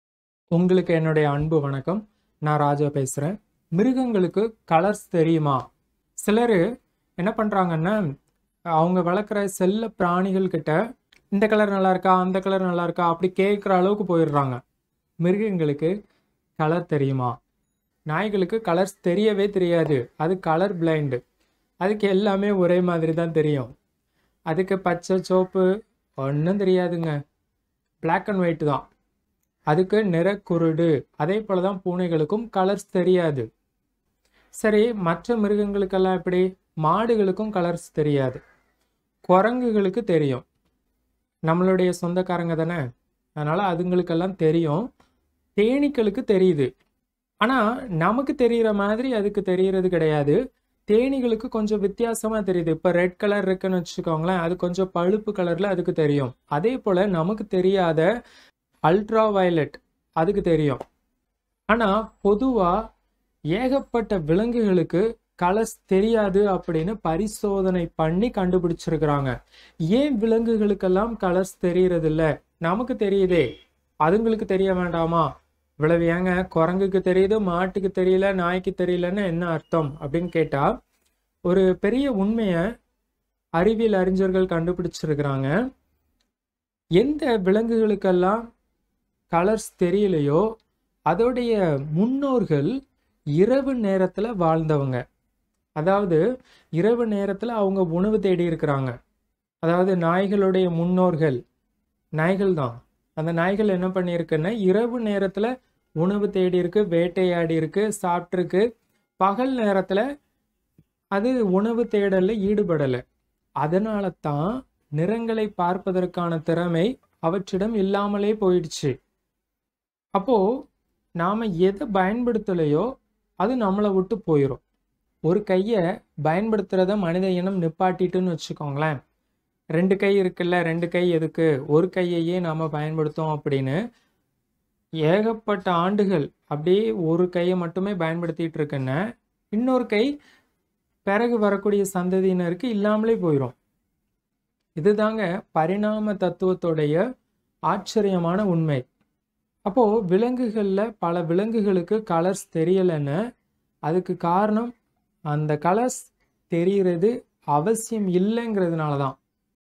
மிருகங்களுக்கு வண்ணங்கள் தெரியுமா ? | Tamil River
Most of the animals we know do not see colors. Cows, dogs and cats do not see colors. Monkeys know colors. Honeybees also know colors. In general, many animals do not see colors. The progenitors of animals without colors hunted at night. For centuries they have hunted and lived only at night. They sleep during the day. So those animals lost the need for colors. Many people mistakenly think that animals know colors. This video aims to dispel that superstition. This video explains scientific facts in a simple way. This video will be useful for those who want to know scientific information.
All human beings in the world should live with love and happiness. My heartfelt congratulations to everyone.